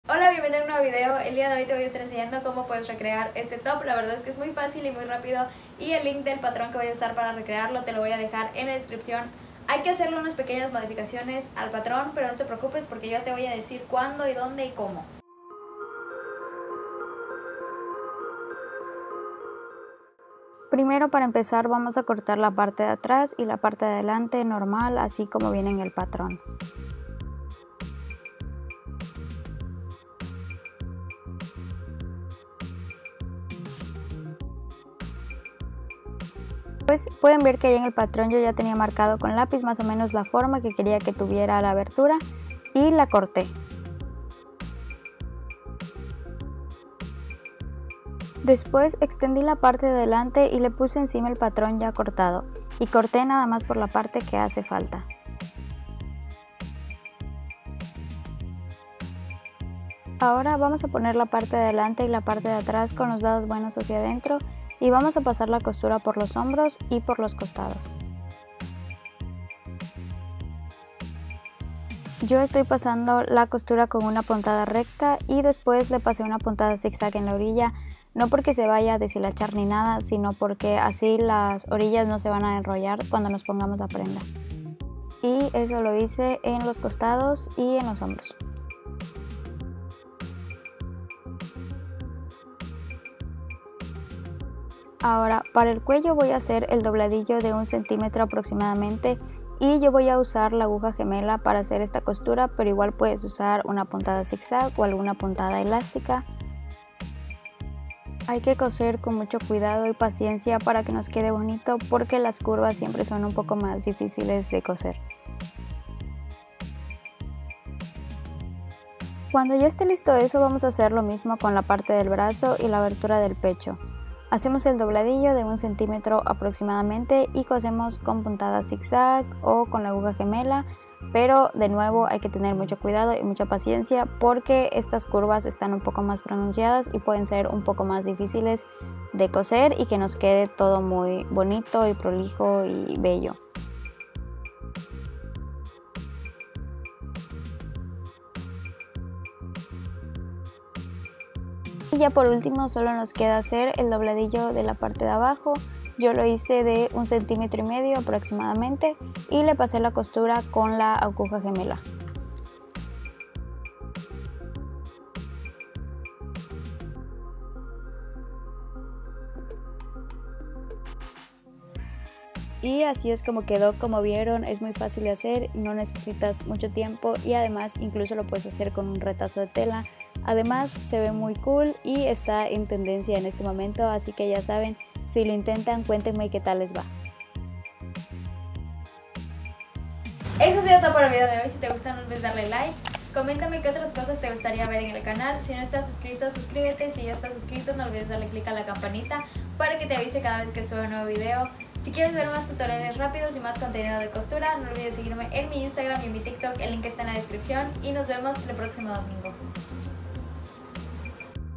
RECREANDO ROPA DE SHEIN (PARTE 5) - Tutorial de top fácil y rápido!!
En el video de hoy les muestro el tutorial de como recrear un top de shein fácil y rápido y también les dejo el patrón gratis del que se pueden guiar para hacerlo!!

*en el pdf hay 3 patrones diferentes, estoy usando el Eva Top (página 7 a 12)*